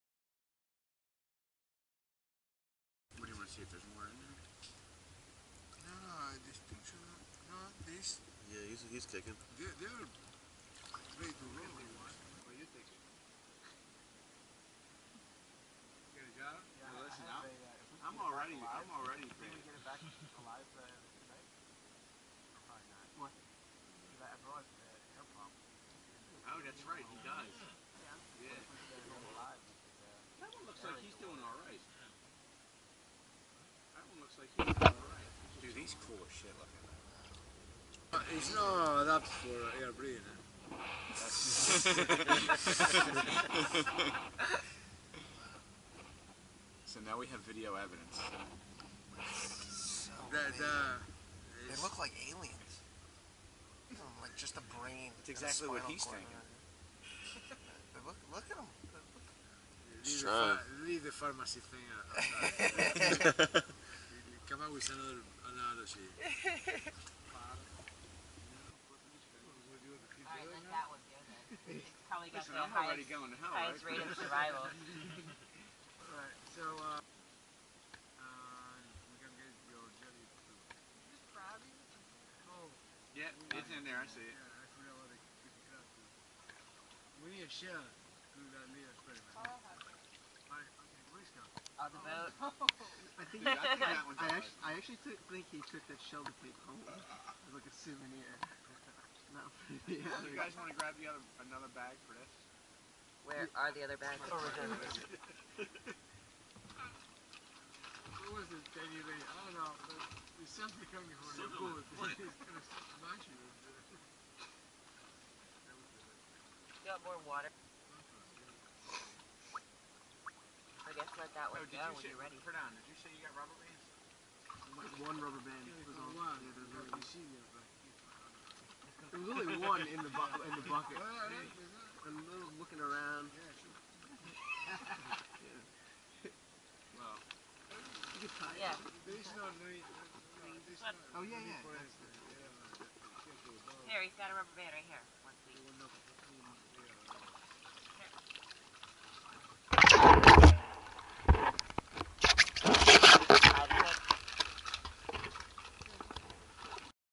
Marine Field Ecology: Mangrove Moment
Exploration of Mangrove Island in Looe Key.